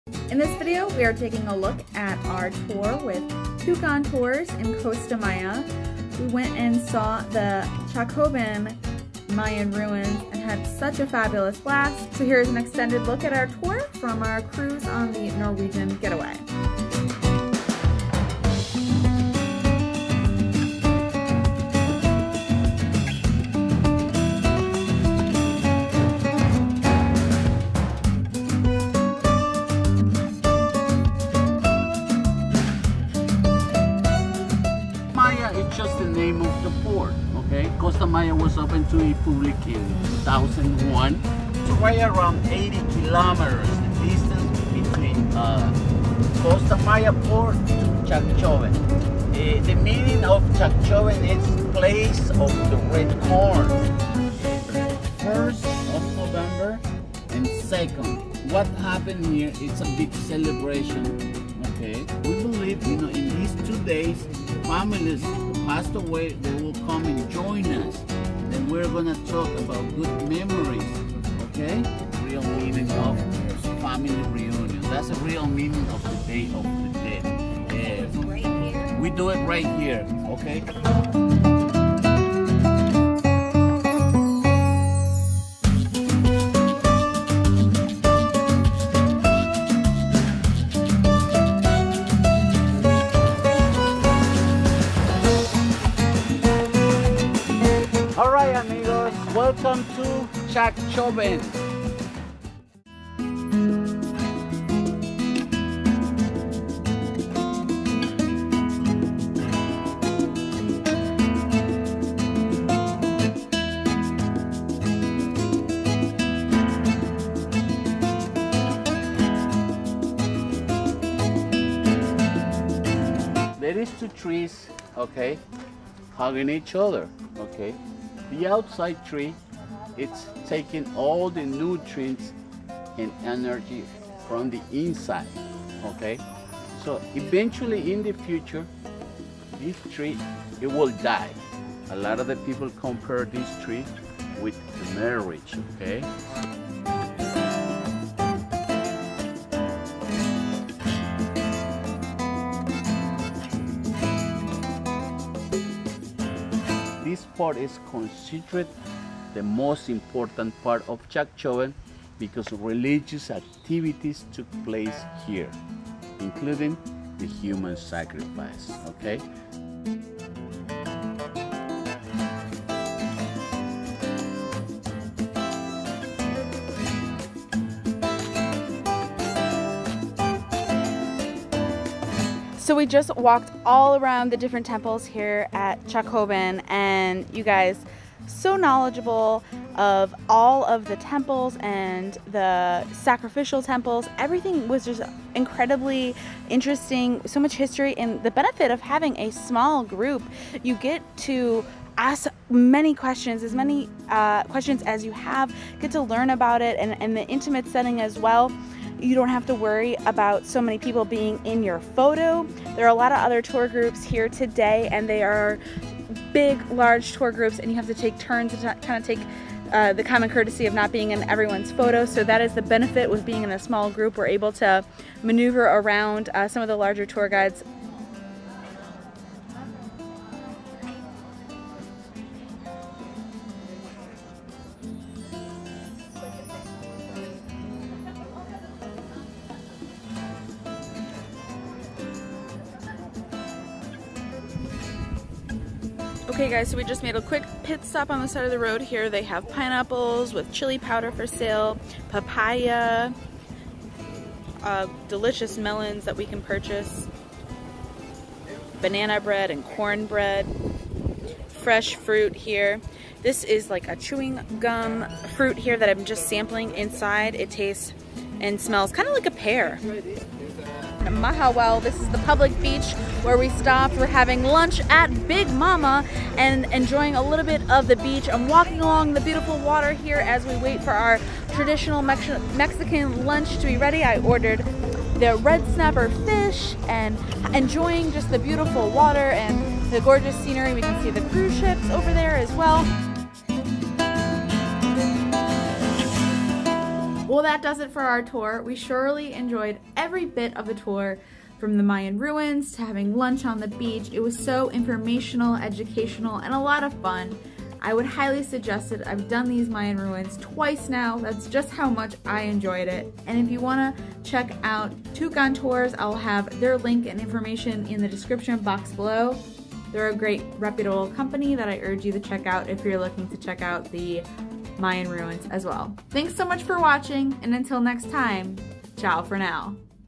The BEST Mayan Ruin Tour in COSTA MAYA to Chacchoben
The best excursions to visit the Mayan ruins of Chacchoben are with Tucan Tours!  While we visited the cruise port of Costa Maya, Mexico, we took a tour that was customizable and intimate in size!
We loved learning all of the history of the Mayan culture and exploring the temples! After our excursion, to the ruins, we headed to Manahual beach and enjoyed an incredible beach lunch!